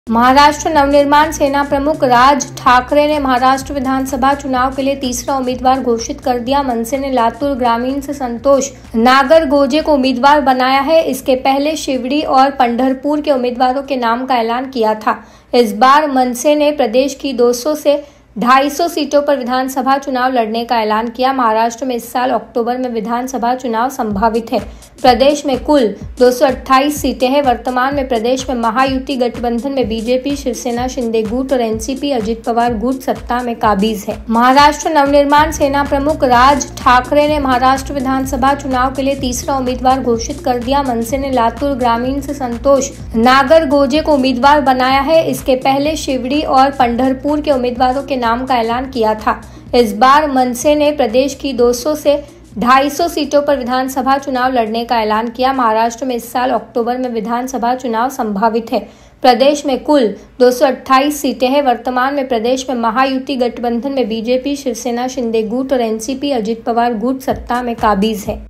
Raj Thackeray ने महाराष्ट्र विधानसभा चुनाव के लिए एक और उम्मीदवार का किया ऐलान
महाराष्ट्र नवनिर्माण सेना प्रमुख राज ठाकरे ने महाराष्ट्र विधानसभा चुनाव के लिए तीसरा उम्मीदवार घोषित कर दिया है. मनसे ने लातूर ग्रामीण से संतोष नागरगोजे को उम्मीदवार बनाया है. इसके पहले शिवडी और पंढरपुर के उम्मीदवारों के नाम का ऐलान किया था.इस बार महाराष्ट्र नवनिर्माण सेना (MNS) ने प्रदेश की 200 से 250 सीटों पर विधानसभा चुनाव लड़ने का ऐलान किया है. महाराष्ट्र में इस साल अक्तूबर में विधानसभा चुनाव संभावित हैं. प्रदेश में कुल 228 सीटें हैं. वर्तमान में प्रदेश में महायुति गठबंधन में बीजेपी, शिवसेना शिंदे गुट और एनसीपी अजित पवार गुट सत्ता में काबिज हैं.